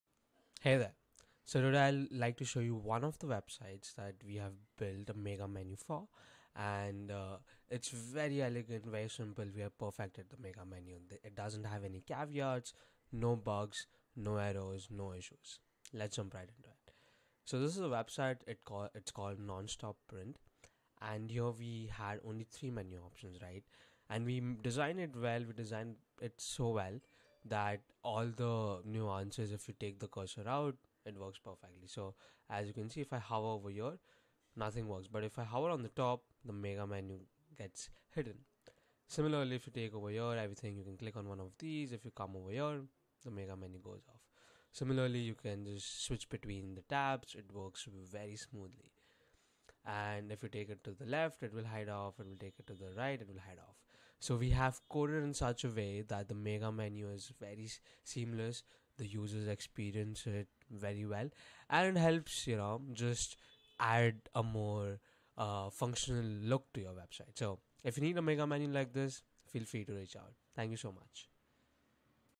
Hover Mega Menu | Wix Velo | Wix Dev
We have designed an elegant mega menu that enhances the user experience by providing a smooth and seamless navigation experience. The mega menu is visually appealing and easy to use, making it an effective tool for guiding users through your website.